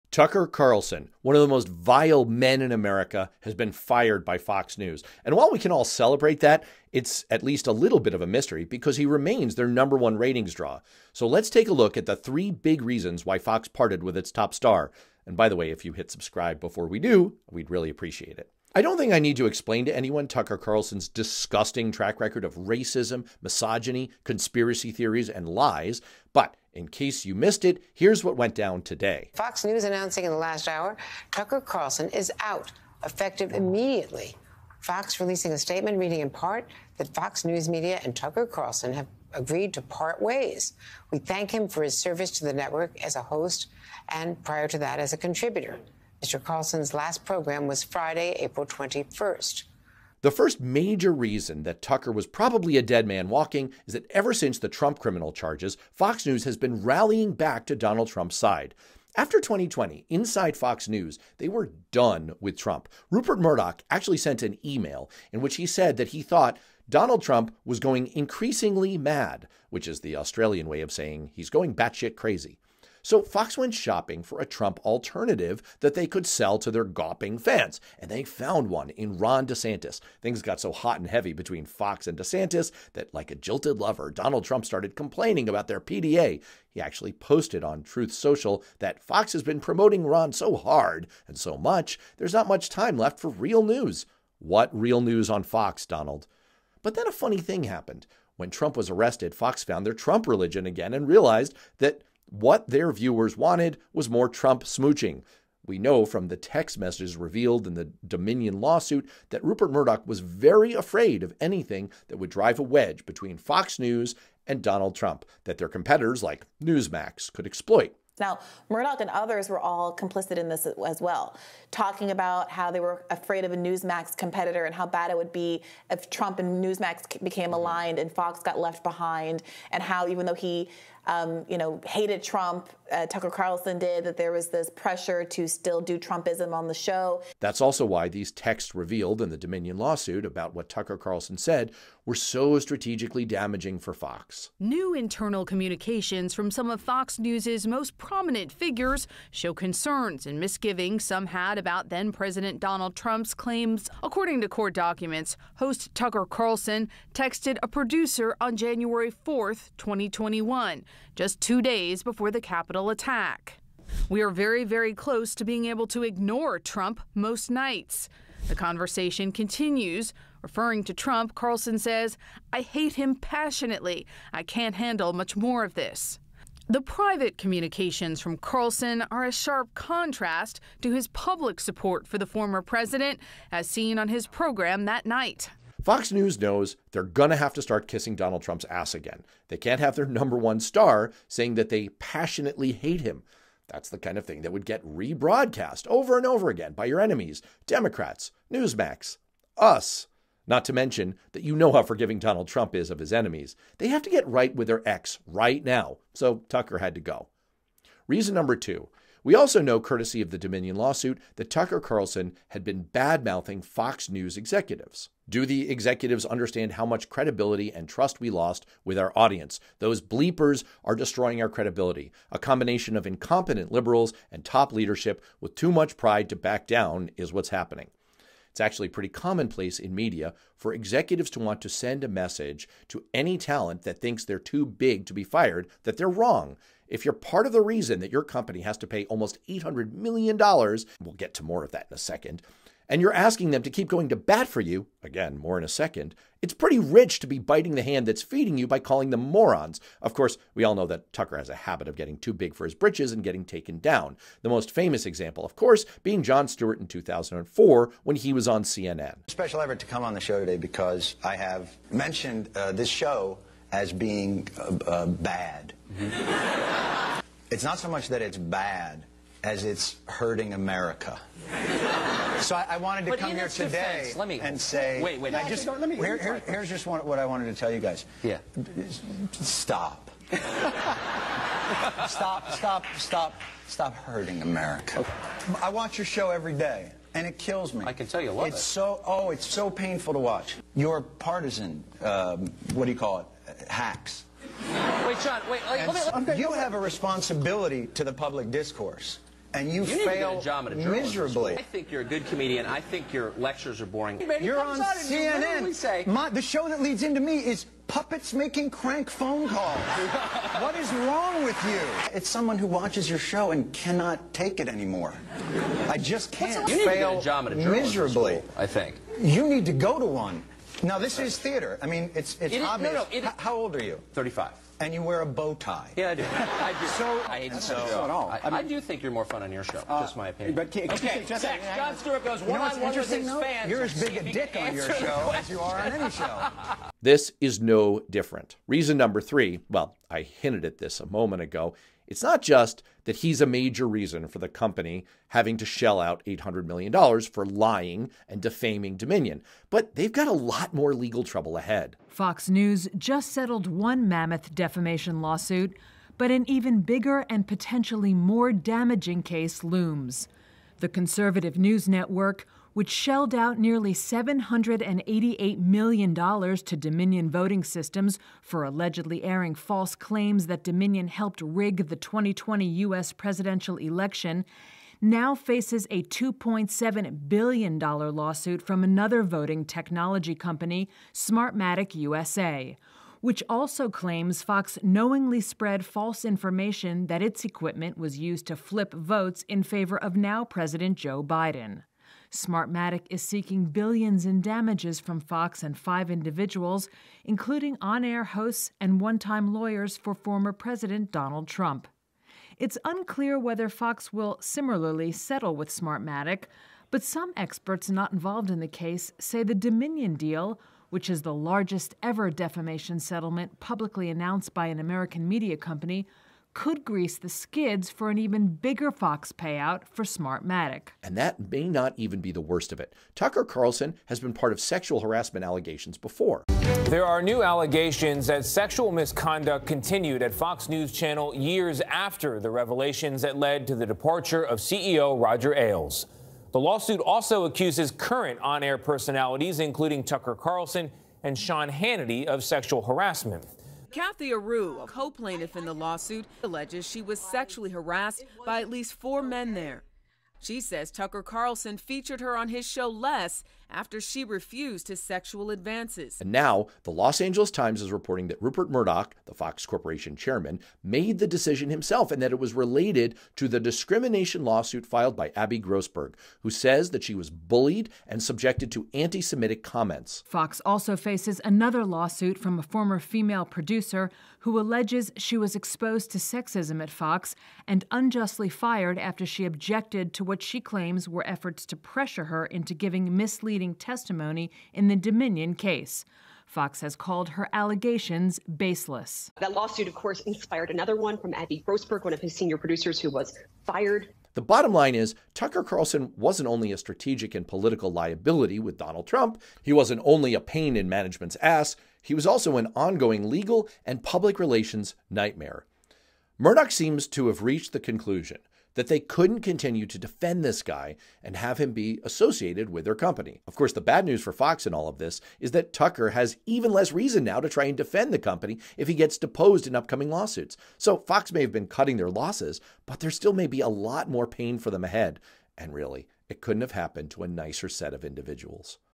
Three Clues Why Right Wing Media Lunatic Tucker was Fired
Vile right-wing Fox News host Tucker Carlson was fired...but why? He was still Fox's number one ratings draw.  We go through the three major reasons that caused Fox News Chairman Rupert Murdoch to decide to Jettison Carlson.

0:00 Tucker Carlson fired
0:52 The Trump Problem
2:21 Devastating messages revealed
3:34 Dissing Fox Execs
4:14 The epic Jon Stewart takedown
5:49 PR Nightmare Ahead
7:13 Harassment
8:30 The Bottom Line

🤣🤩🤯 GO PURCHASE THEIR AWESOME SWAG RIGHT NOW!